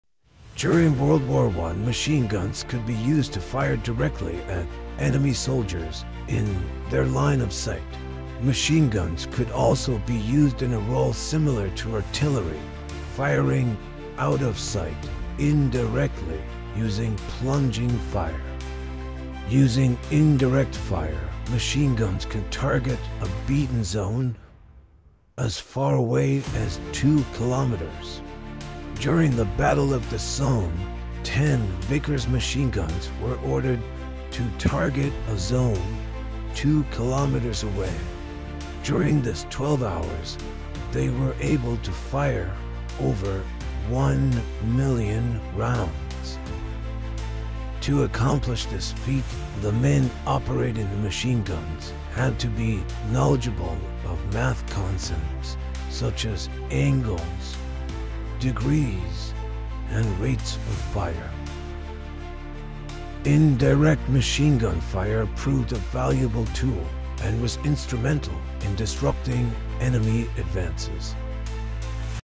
By using indirect fire MG units of WW1 could fire at out of targets well beyond the line of sight
These newsreel clips show MGs elevated and firing their weapons using indirect fire or plunging fire. This role was similar to artillery. Using calculations involving angles and distance the MGs could target what is called a "beaten zone" often as far as 2 kilometers away not in direct line of sight. The soldiers operating MGs and using indirect fire had to be knowledgeable of math and required special training.

Indirect fire

Colourized by cassowarycolorizations